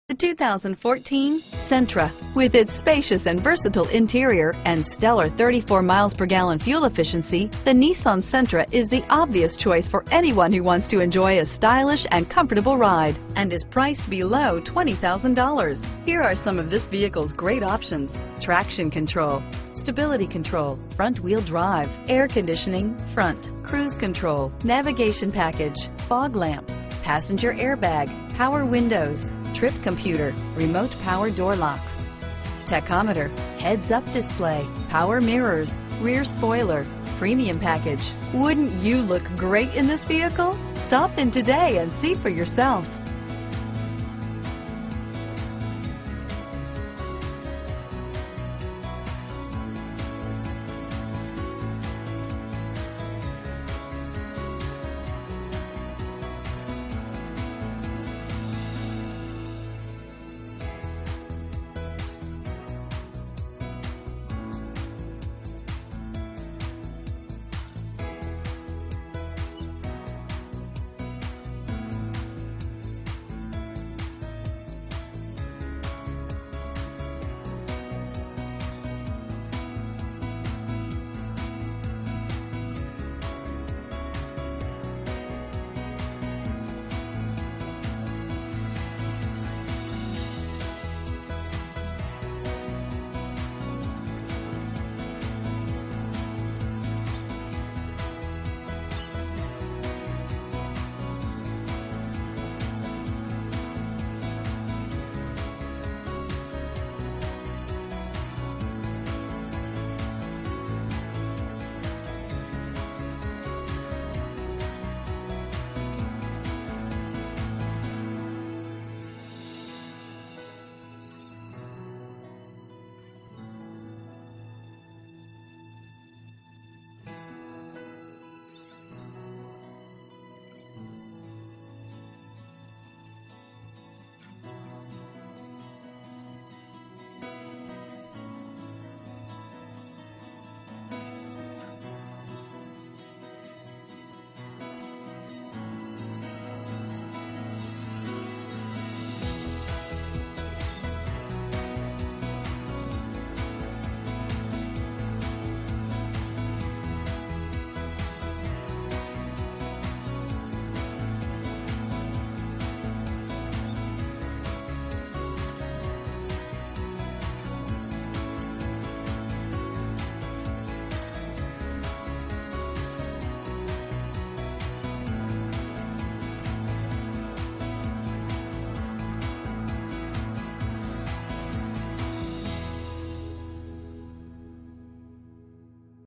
2014 Nissan Sentra Morton Grove, Des Plaines, Glenview, Evanston, Mt Prospect, IL 21520
2014 Nissan Sentra

Star Nissan
5757 W Touhy Ave

2014NissanSentraSR1.8L 4 cylsCVT (Continuously Variable)Magnetic GrayCharcoal Optional equipment includes: Premium Package, SR Driver Package, Navigation Package, Carpeted Floor Mats w/Trunk Mat...Standard features include: Remote power door locks, Power windows with 1 one-touch, CVT Transmission, 4-wheel ABS brakes, Air conditioning, Cruise control, Audio controls on steering wheel, Traction control - ABS and driveline, Tilt and telescopic steering wheel, Multi-function remote - Trunk/hatch/door/tailgate, Power mirrors, Head airbags - Curtain 1st and 2nd row, Passenger Airbag, 1.8 liter inline 4 cylinder DOHC engine, 4 Doors, Front-wheel drive, Fuel economy EPA highway (mpg): 39 and EPA city (mpg): 30, Front fog/driving lights, Tachometer, External temperature display, Interior air filtration, Overhead console - Mini with storage, Rear spoiler - Lip, Clock - In-radio display, Stability control, Trip computer...Safety equipment includes: ABS, Traction control, Curtain airbags, Passenger Airbag, Front fog/driving lights...Other features include: Power locks, Power windows, CVT Transmission, Air conditioning, Cruise control...